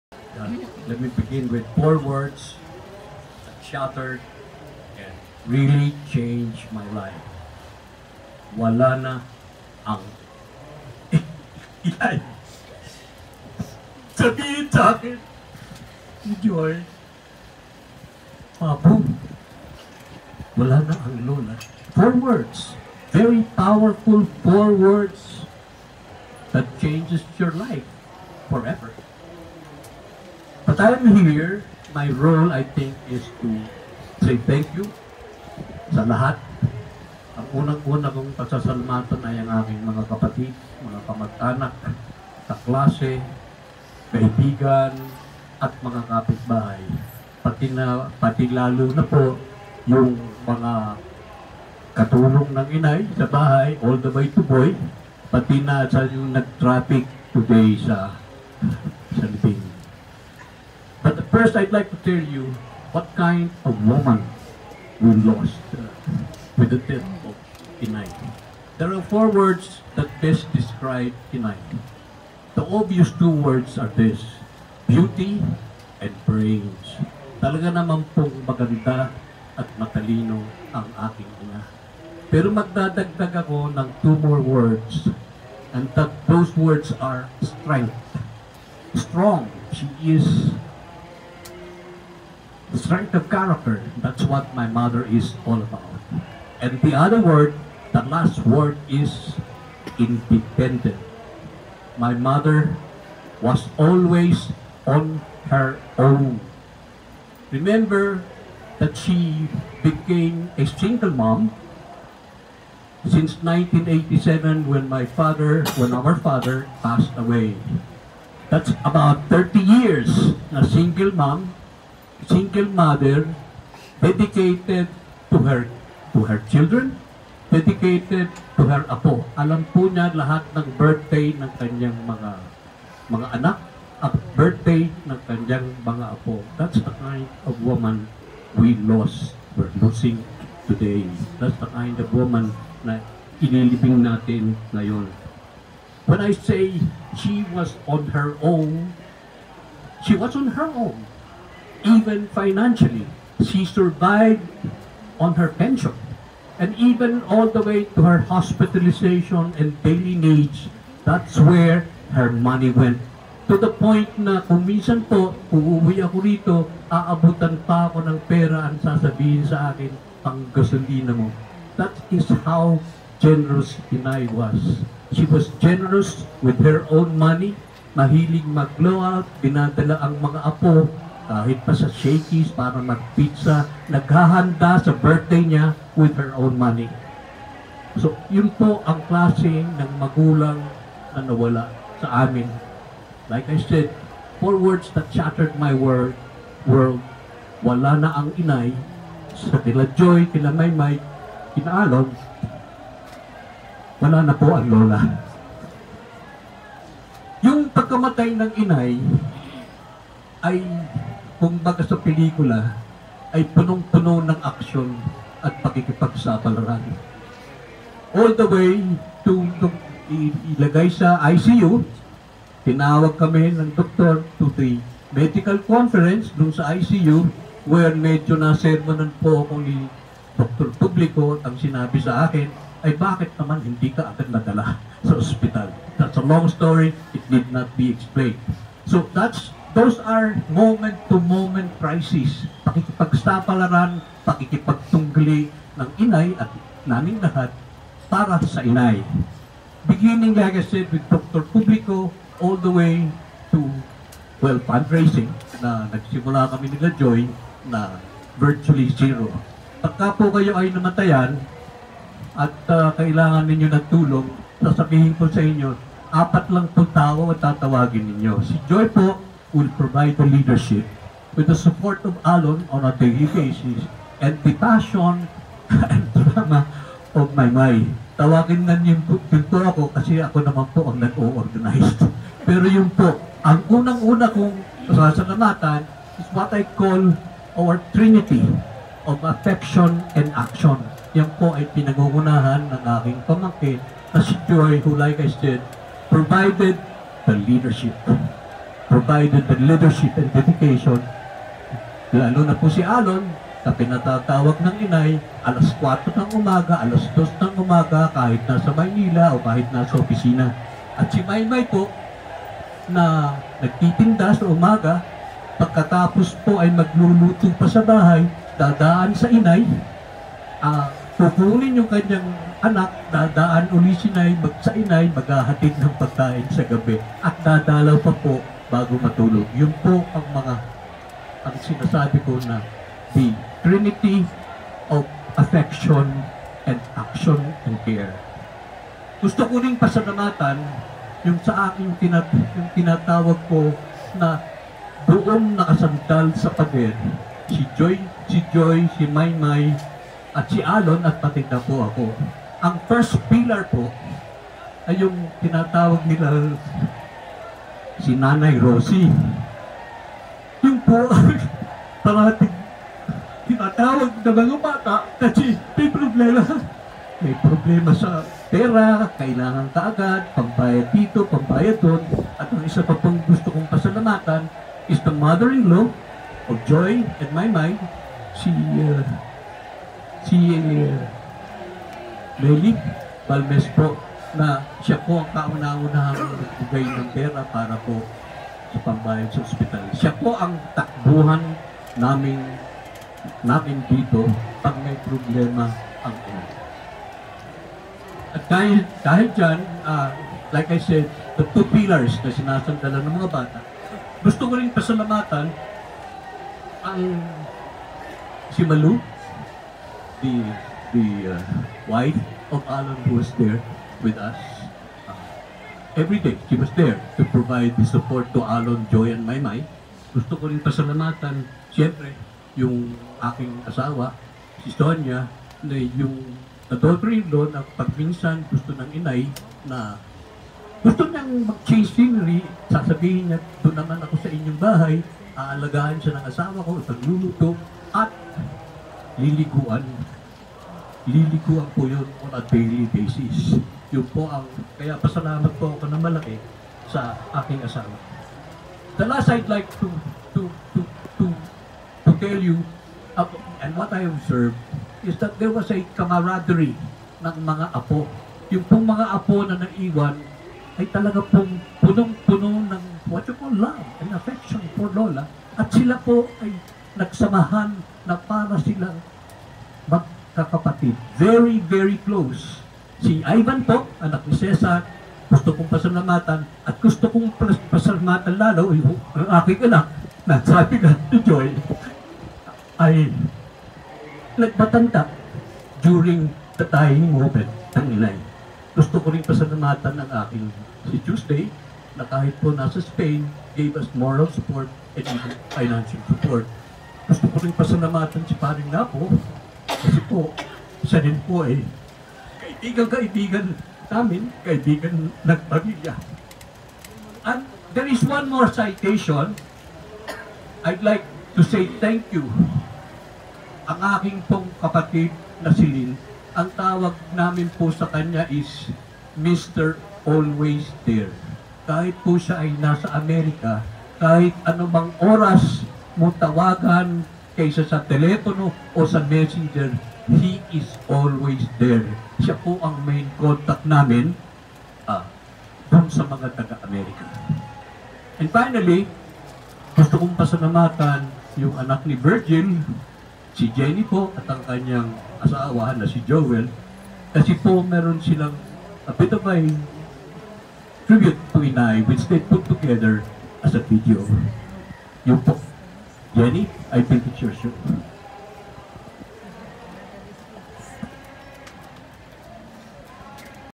Tito Manny’s eulogy for Lola Concha 💜
2/18/2018
Paalam, Lola Concha! ✝️💜💐💟👵🏼